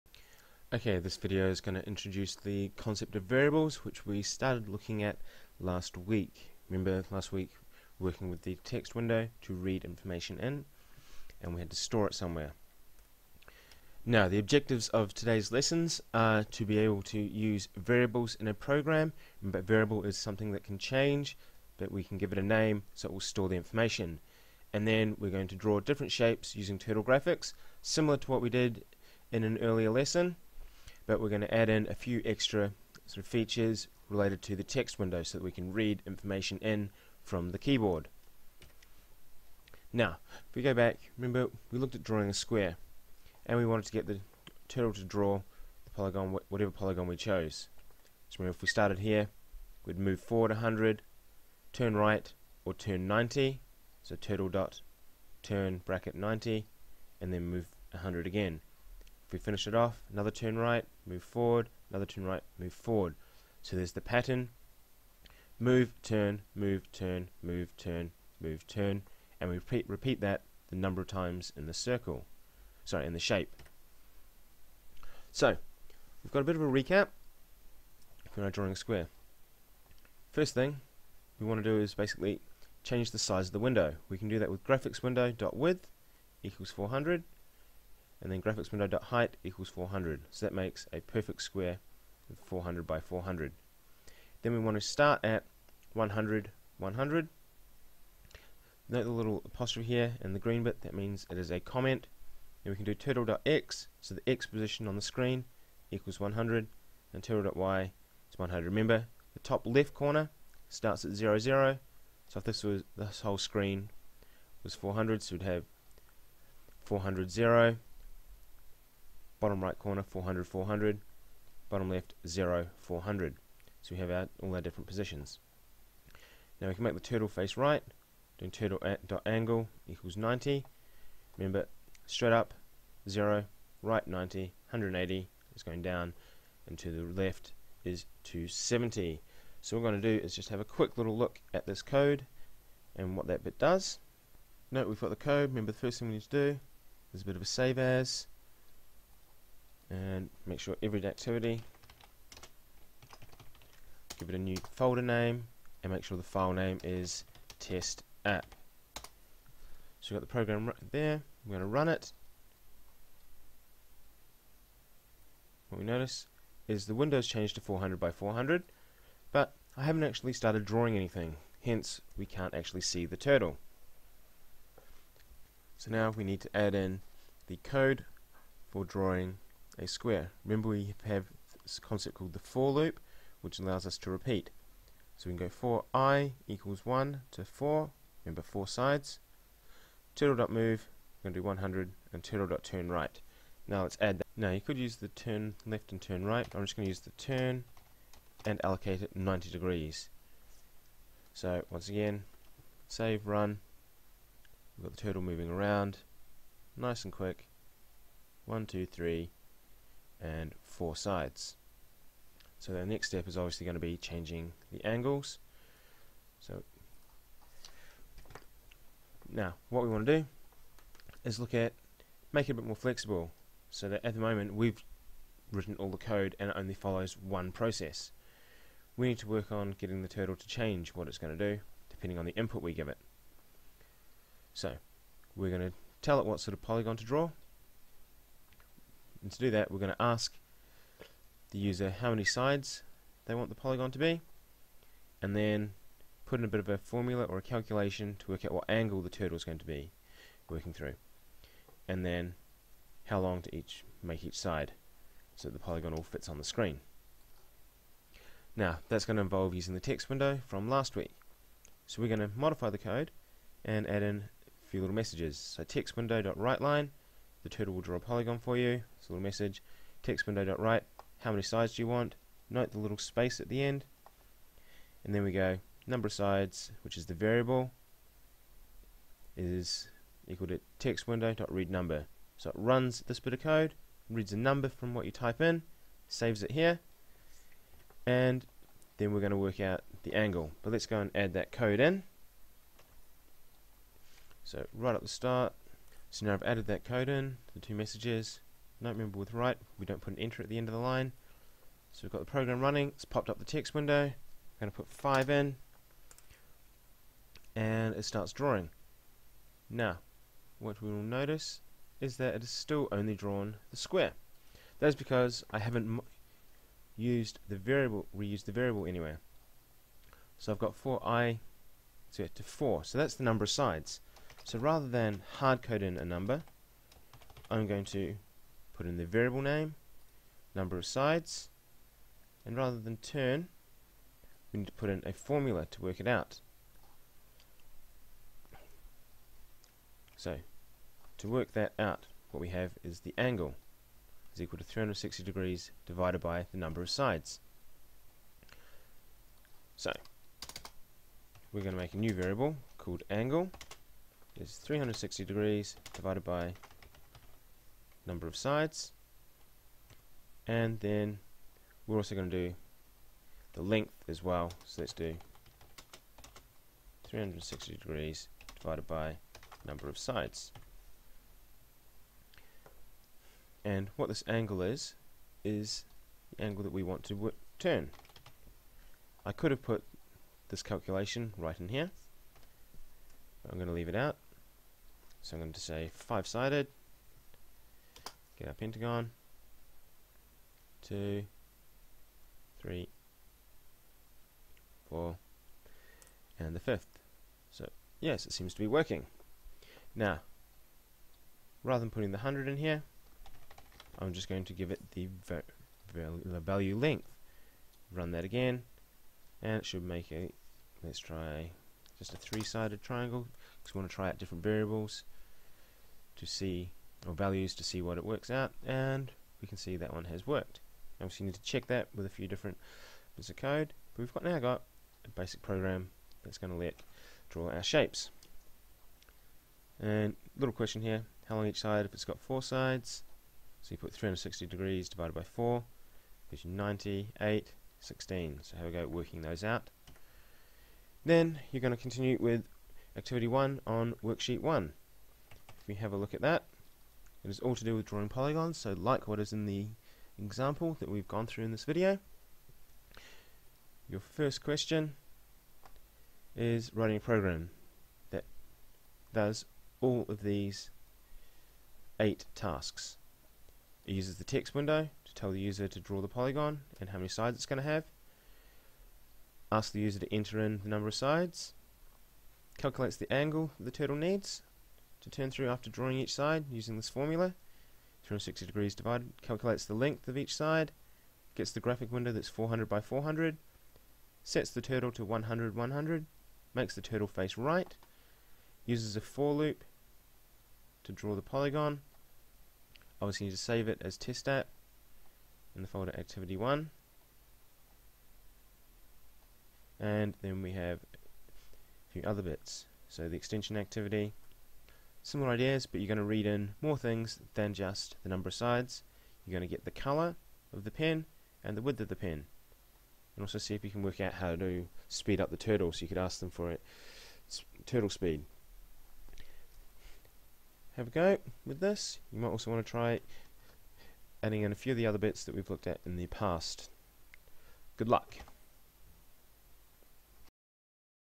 Small Basic L4 Using Variables Shapes again
Tutorial on how to use the Text Window and Graphics window to draw shapes based on user input.